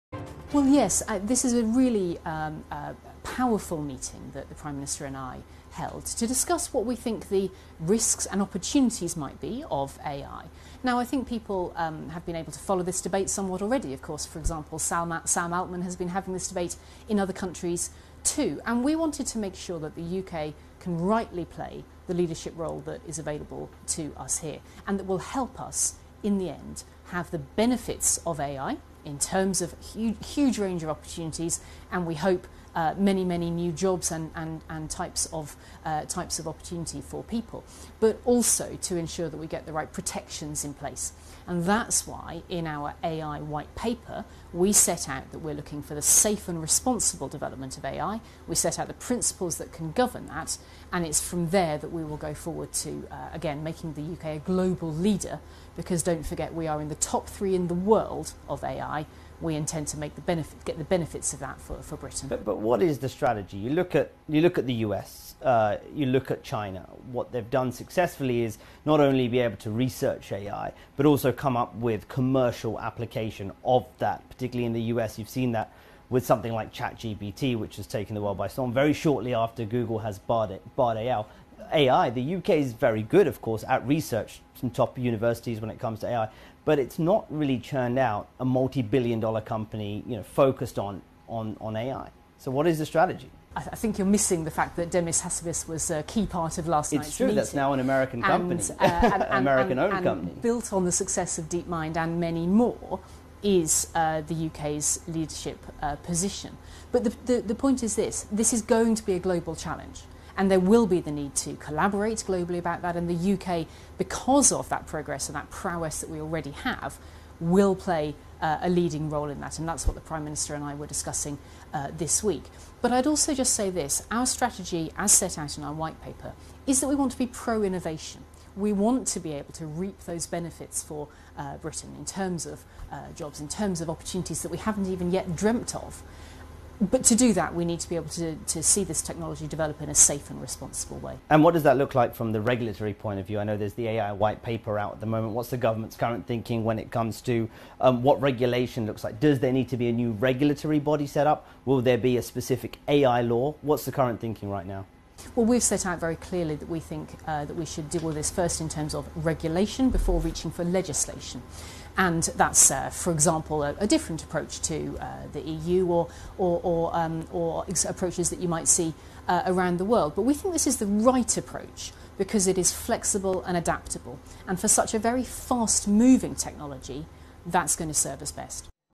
UK aims to be a 'global leader' in A.I., tech minister says
U.K. Science, Innovation & Technology Secretary Chloe Smith discusses the country's strategy to bolster its AI industry.